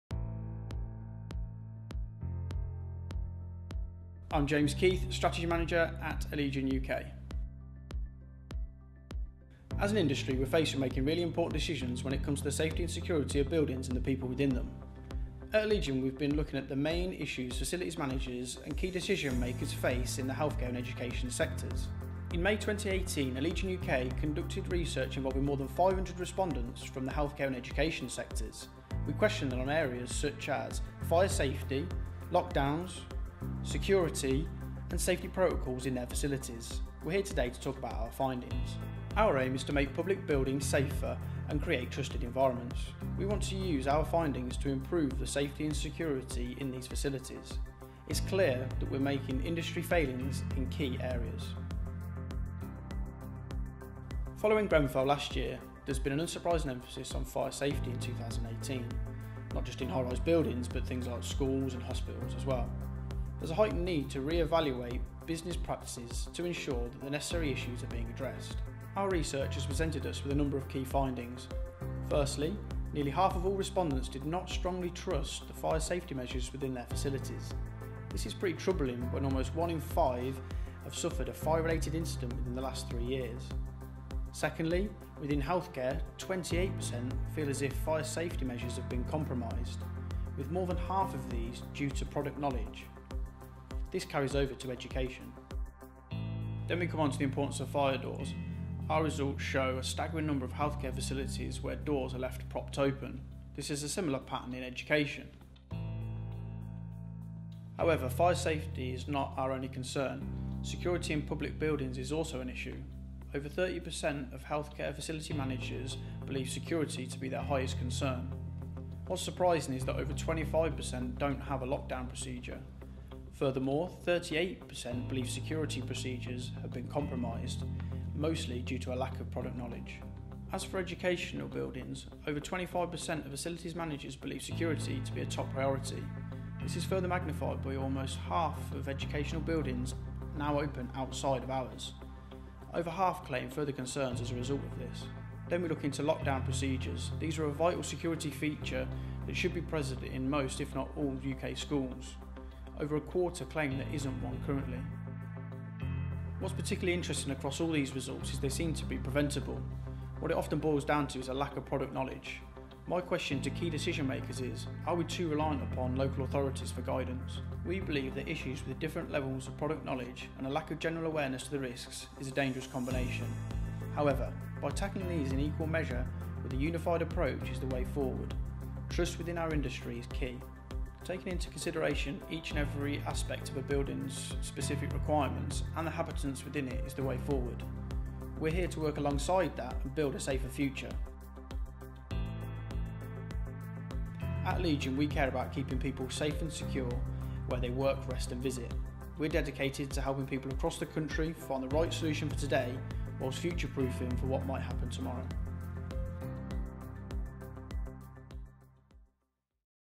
Allegion reviews UK Fire Safety and Security Regulation issues in Healthcare and Education.
The healthcare and education sectors are continuously facing safety and security concerns within their premises. This comes as a result of obligations to remain compliant to codes and regulations, whilst also balancing budgeting pressures or other restricting internal issues.